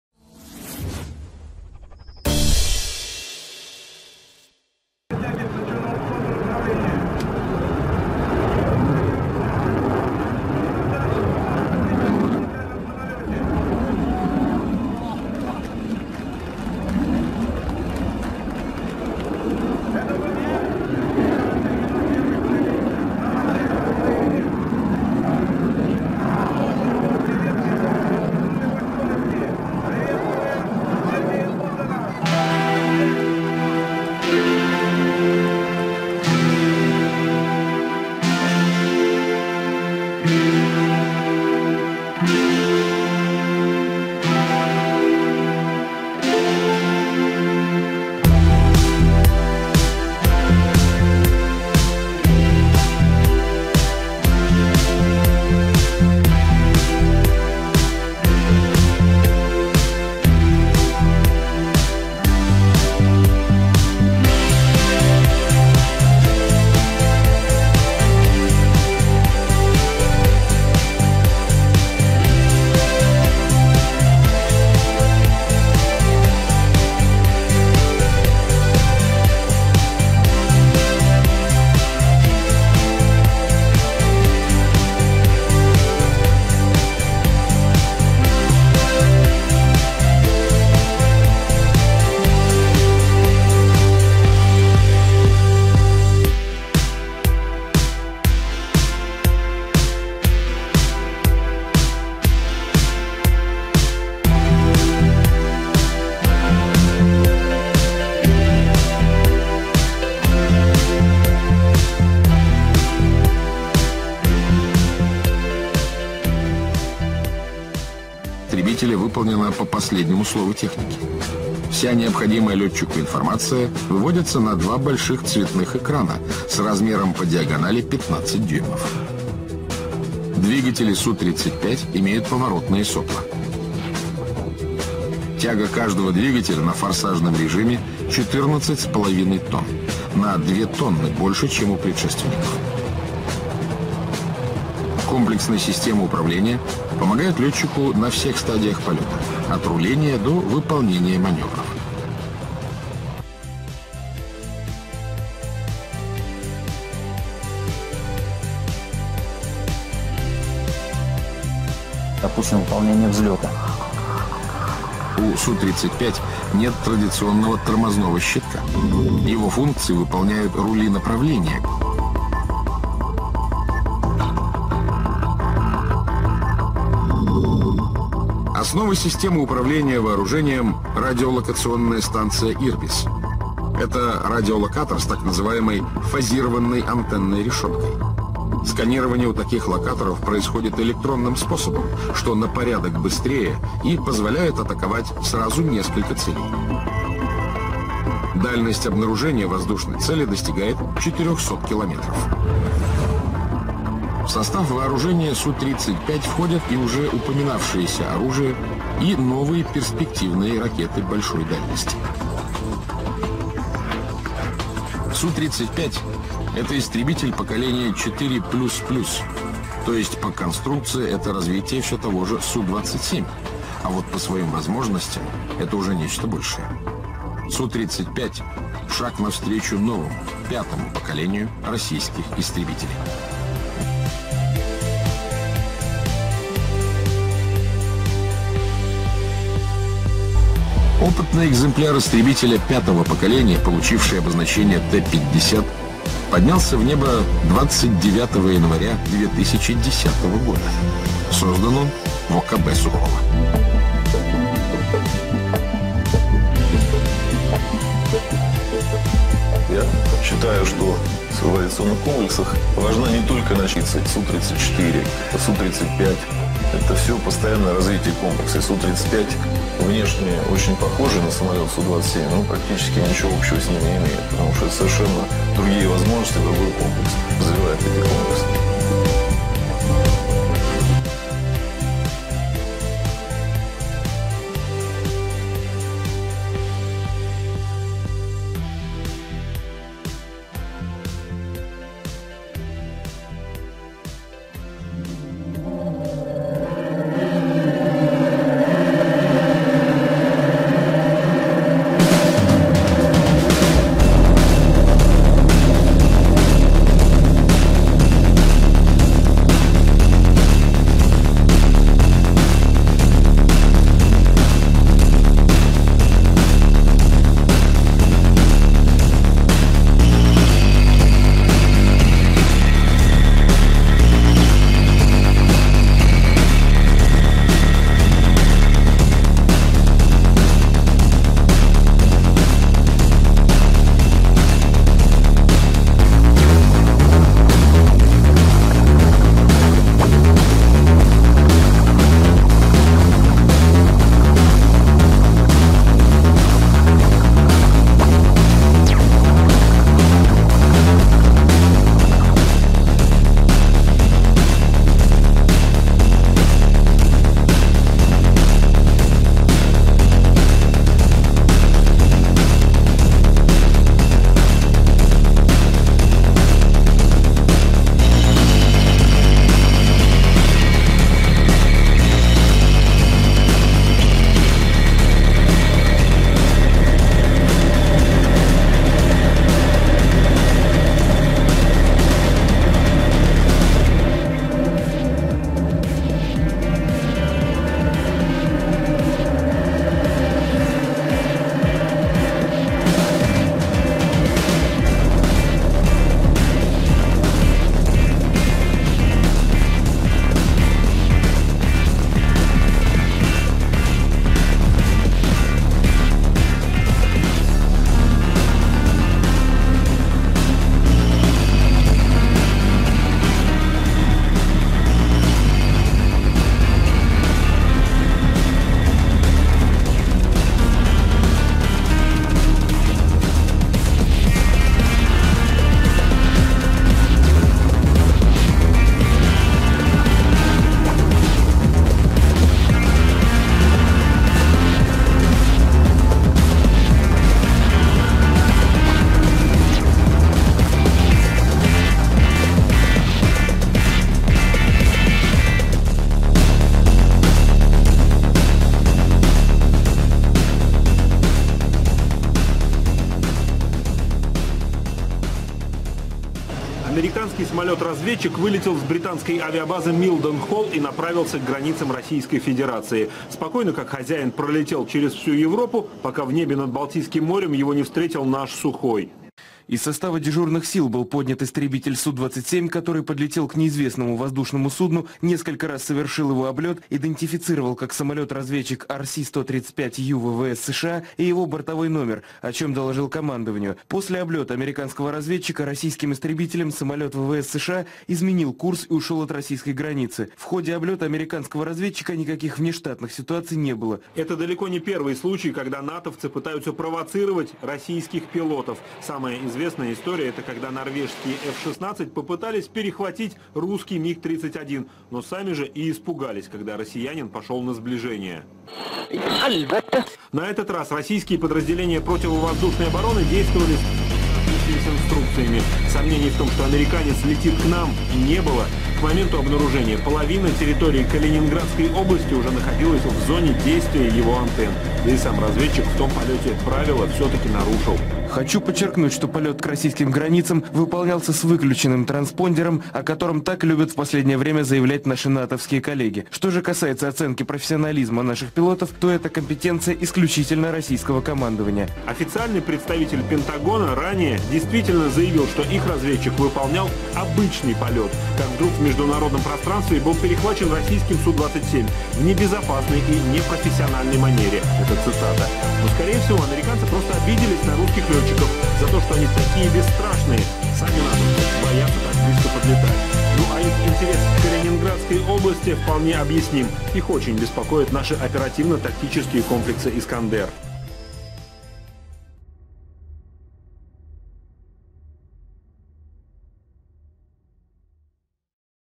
Надо знать SU-27, СУ-35. Это ВВС России.
Почему надо знать и помнить о боевом самолете SU-35, SU-27.  вы узнаете из этого видео.